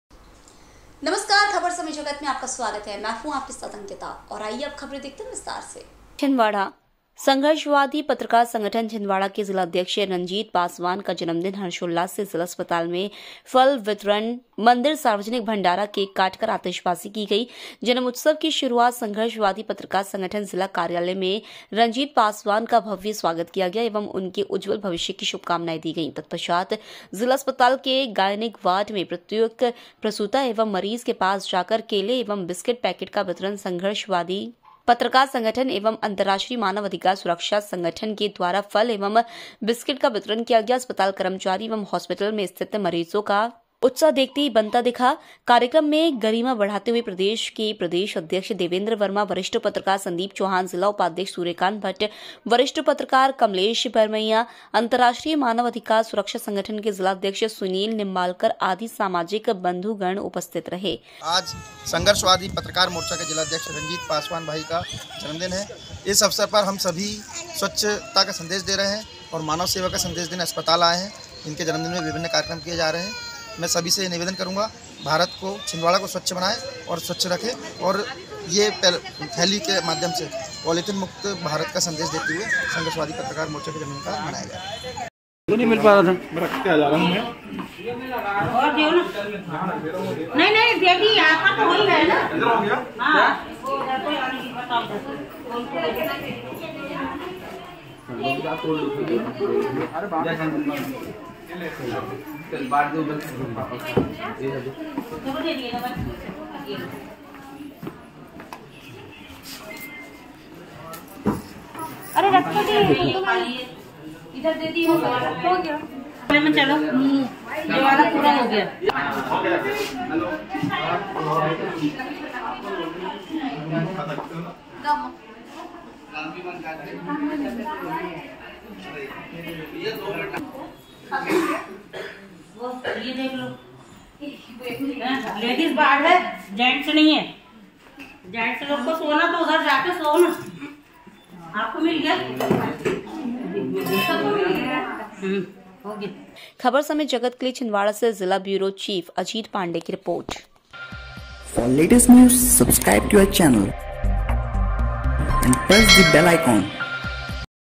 संघर्षवादी पत्रकार संगठन जिलाध्यक्ष रंजीत पासवान का जन्मदिन हर्षोल्लास से मनाया गया
जिला ब्यूरो चीफ अजीत पांडे की रिपोर्ट